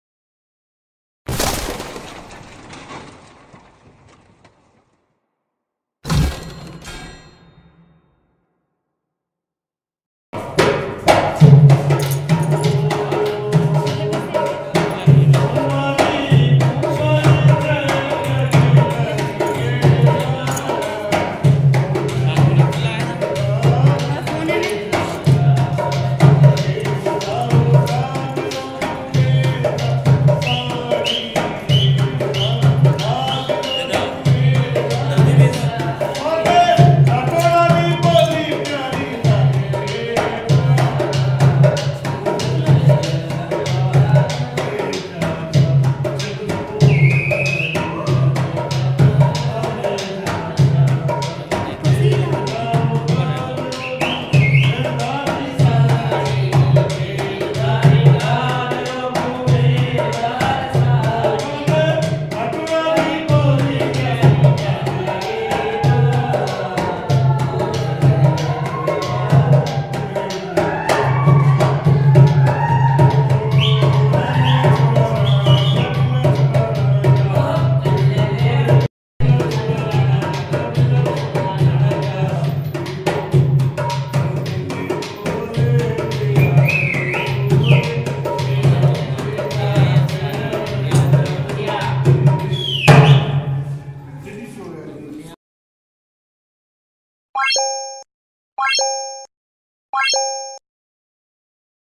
The Puppet Show.||Amber or Amer fort.
Kathputli is a string puppet theatre, native to Rajasthan, India, and is the most popular form of Indian puppetry. Being a string marionette, it is controlled by a single string that passes it from the top of the puppet over the puppeteers.